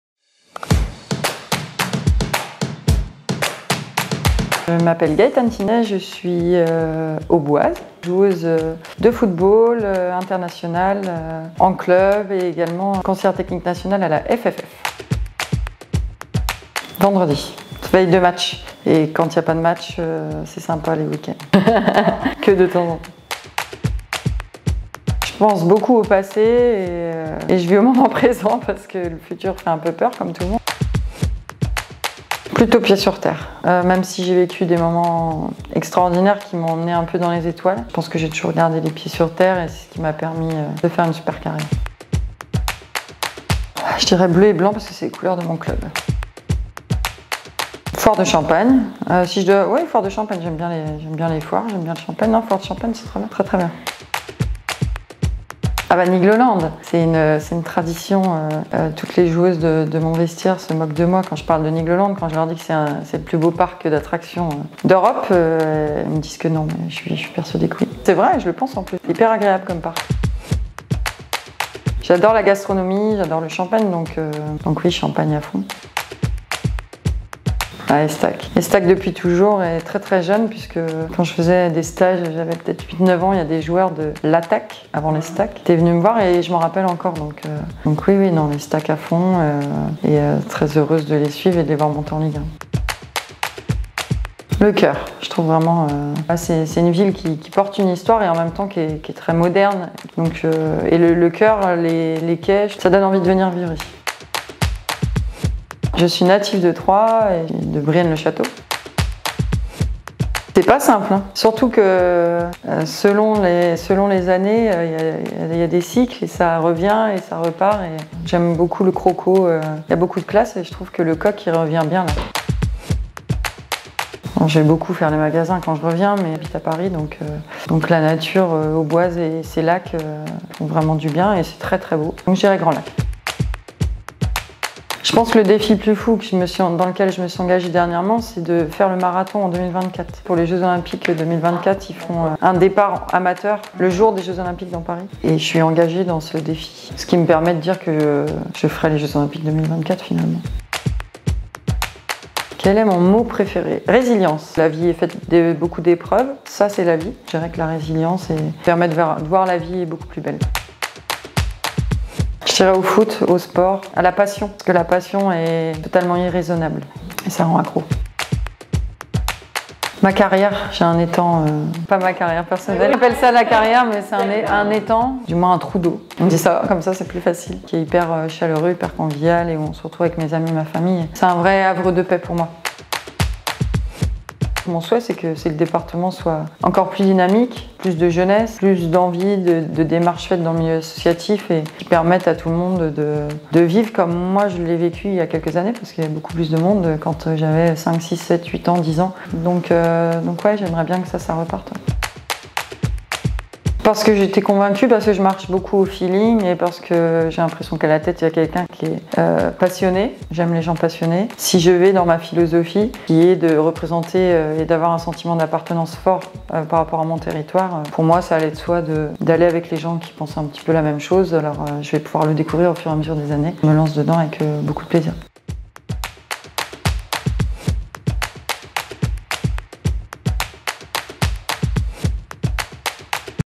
TROMBINI Gaëtane Thiney
Le question-réponse AUBASSADEURS par Gaëtane Thiney, Joueuse de Football Professionnel - FFF .

AUBASSADEURS

Anne-claire & Laurent Vittenet

_CREDITS_